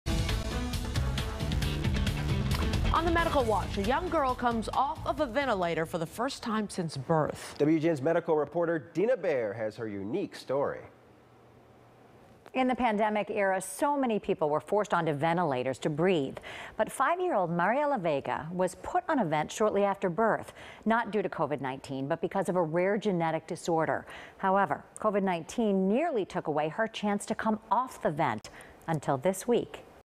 Backpack gives girl freedom from ventilator
In the pandemic era, so many people are forced onto ventilators to breathe. But 5-year-old Mariella Vega was put on a vent shortly after birth, not due to COVID-19 but because of a rare genetic disorder.